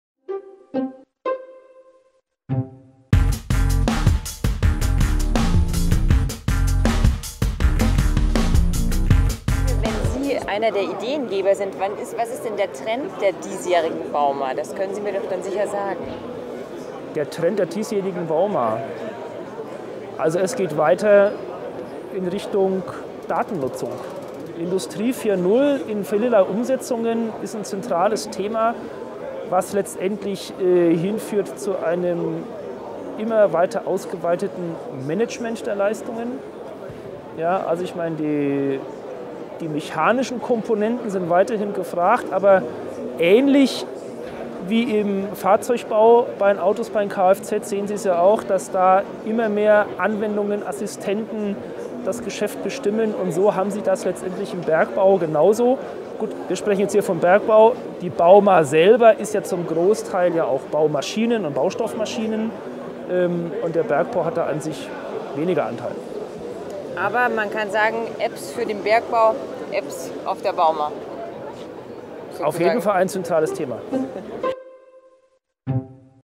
VDMA Mining • Trendthemen bauma
Die VDMA arbeitet eng mit der bauma in München zusammen. Obwohl die bauma erfolgreich ausgebucht ist, leidet die Bergbaubranche unter dem Rohstoffmarkt.

Interview mit Helmut Schgeiner auf der bauma 2016.